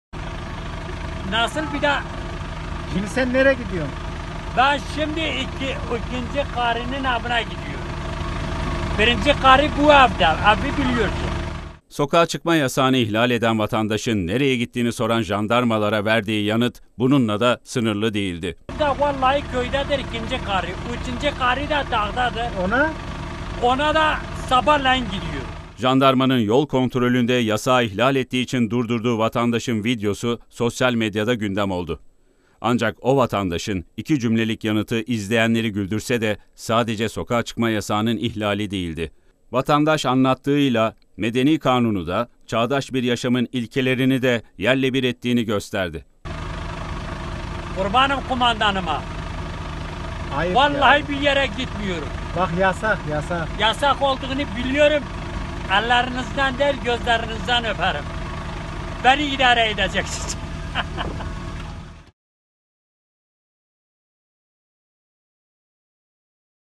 Yasağa rağmen sokağa çıkan vatandaş: İkinci karımın evine gidiyorum
Jandarmanın yol kontrolünde nereye gittiğini sorduğu vatandaşın yanıtı şaşkınlık yarattı. ''İkinci karımın evine gidiyorum, üçüncü karım dağda, sabah da ona gidiyorum'' dedi.

tv100 uydu bilgileri:

TÜRKSAT 4A
Frekans: 12729
Sembol: 30000
Polarizasyon: V (Dikey)